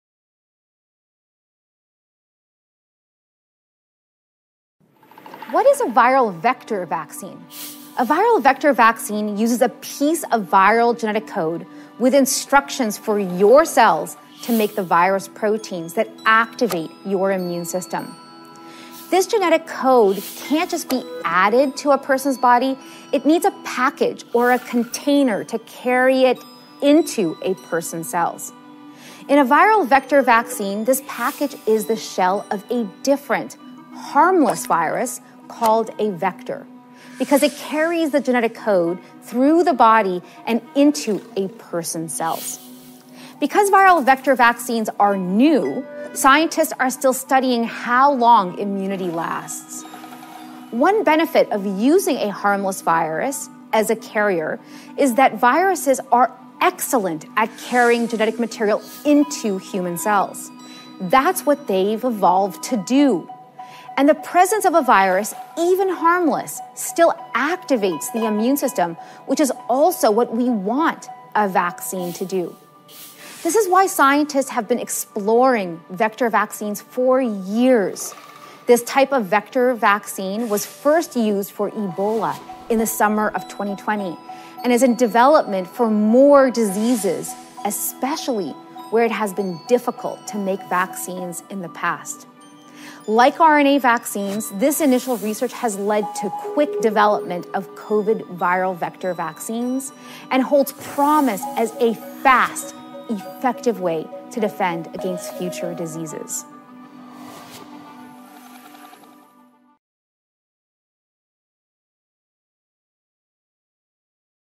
What is a Viral Vector vaccine?
Vaccines help a person’s immune system prevent diseases. Different types of vaccines have different benefits, challenges, and uses. Learn about a new type of vaccine that uses genetic material in a shell made of viruses to activate your immune system.

00:09 What is a Viral Vector vaccine?
00:54 Benefits of this vaccine
01:14 how else is this vaccine used?